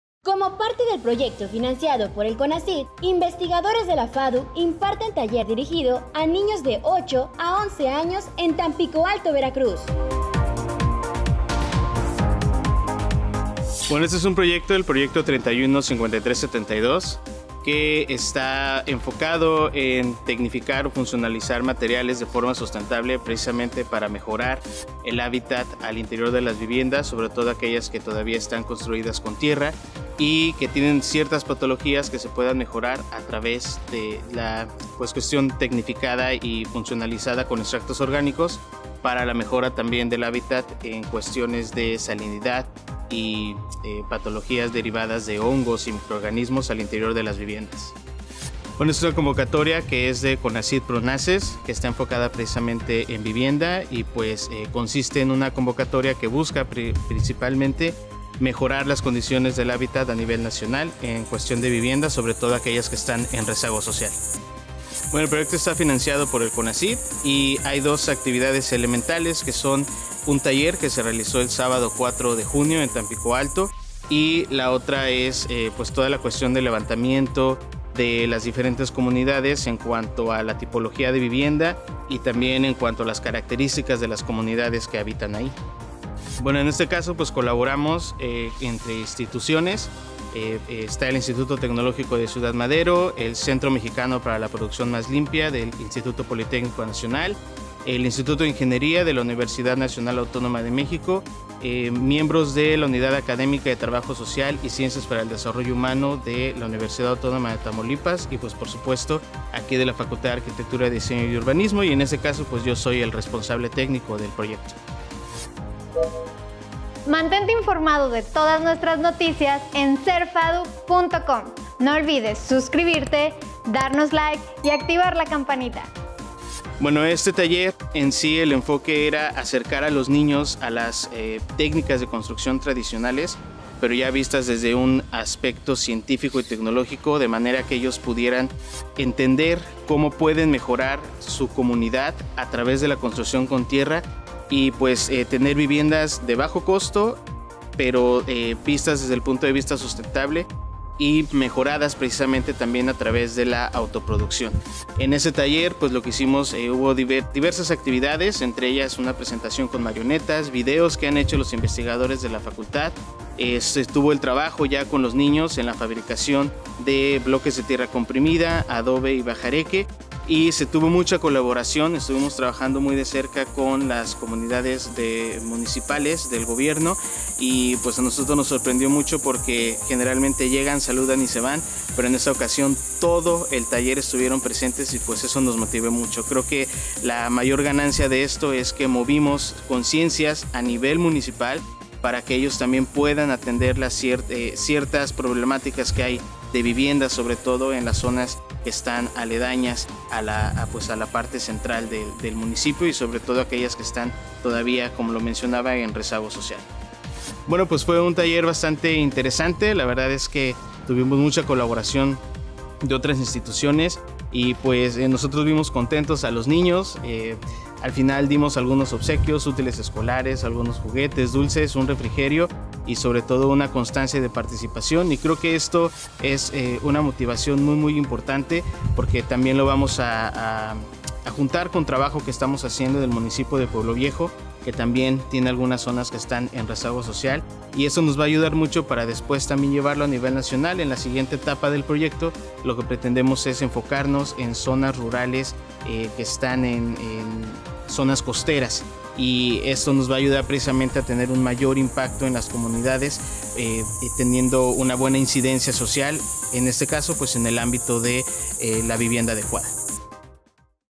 Investigadores FADU imparten taller a niños en Tampico Alto Veracruz
Como parte de las actividades del proyecto “Funcionalización sustentable de materiales tecnificados para la mejora del hábitat rural basados en la construcción con tierra”, con número de convenio 315372 que fue beneficiado en la Convocatoria 2020 “Proyectos de Investigación e incidencia para una vivienda adecuada y acceso justo al hábitat” financiado por el Consejo Nacional de Ciencia y Tecnología (CONACYT), se realizó en el municipio de Tampico Alto, Veracruz el Taller “Rescatando el pasado para construir el futuro”, el pasado 4 de junio.